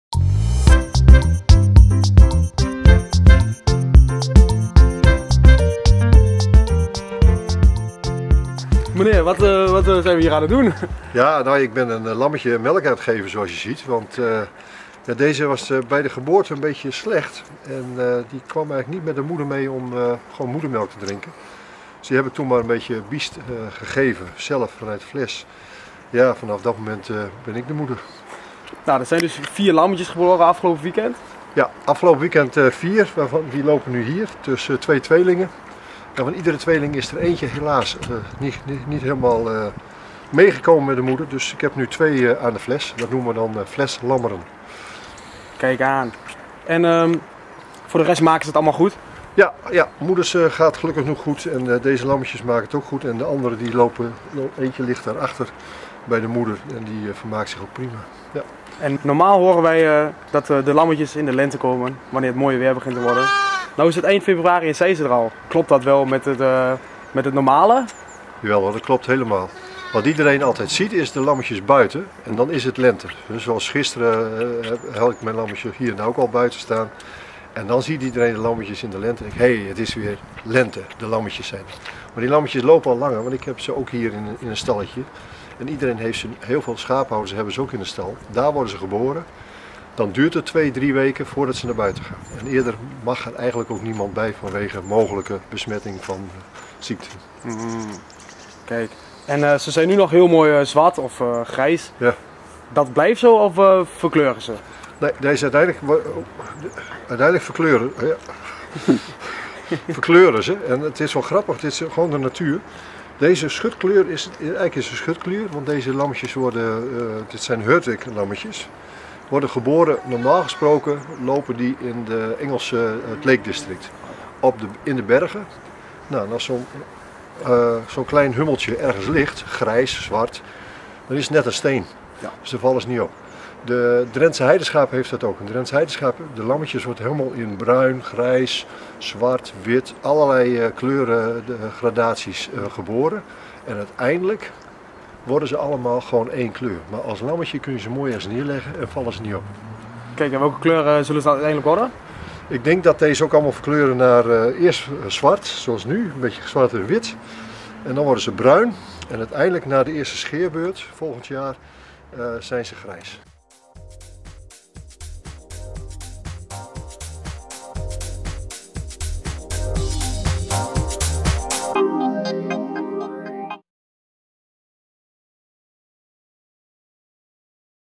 Op kraamvisite bij de stadskudde van Almelo
Er zijn dit weekend 4 lammetjes geboren in Almelo. De stadskudde breidt zich dus weer uit.